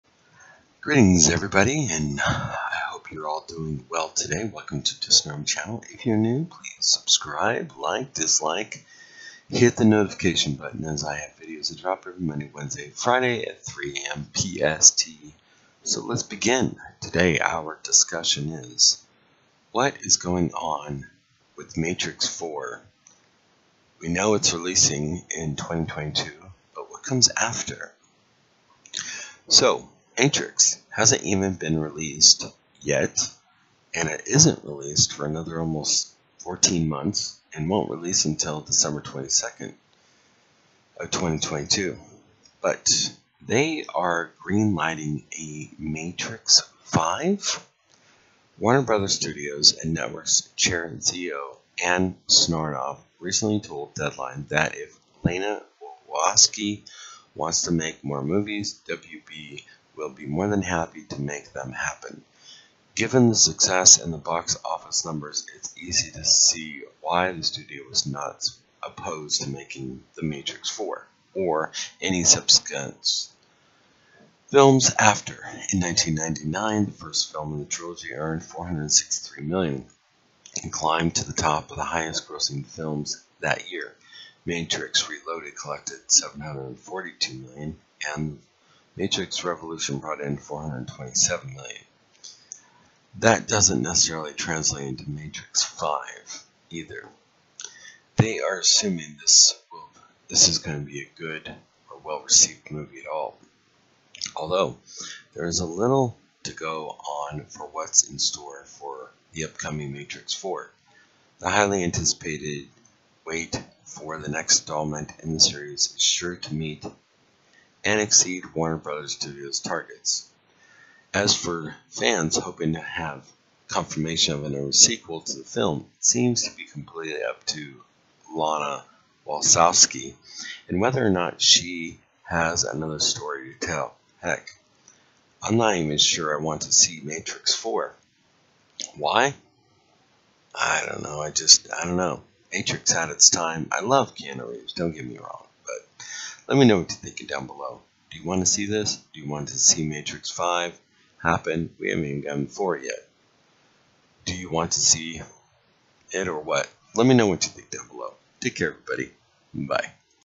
WHAT? Matrix 4 Is Releasing Dec 2022, but will WE be getting more after Matrix 4?
It appears that, it could be the case but nothing is confirmed or in the works, we havent even gotten Matrix 4 yet...